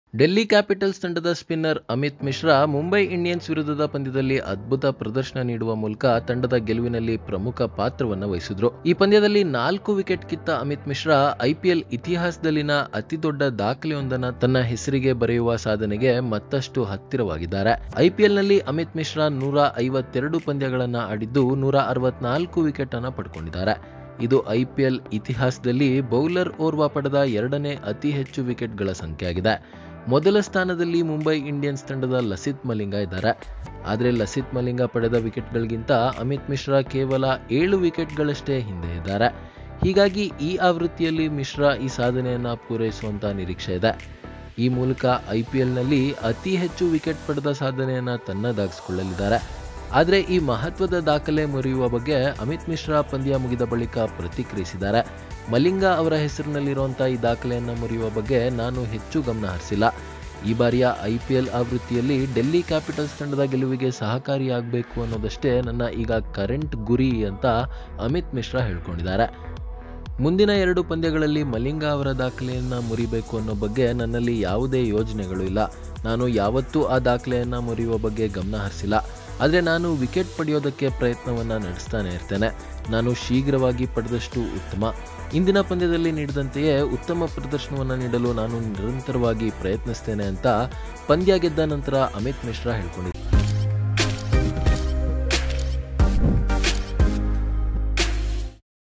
Amith Mishra ಅವರು ಪಂದ್ಯ ಮುಗಿದ ಬಳಿಕ ಹೇಳಿದ್ದೇನು | Oneindia Kannada
ನಿನ್ನೆ 12ನೇ ಪಂದ್ಯದಲ್ಲಿ ಮುಂಬೈ ತಂಡ ಡೆಲ್ಲಿ ತಂಡವನ್ನು ರಾಜಸ್ಥಾನ ಎದುರಿಸಿತು. ಪಂದ್ಯದ ಸಂಪೂರ್ಣ ವಿವರ ಇಲ್ಲಿದೆ

yesterday mi played dd in the 12th match of the ipl. Here are some important updates of the match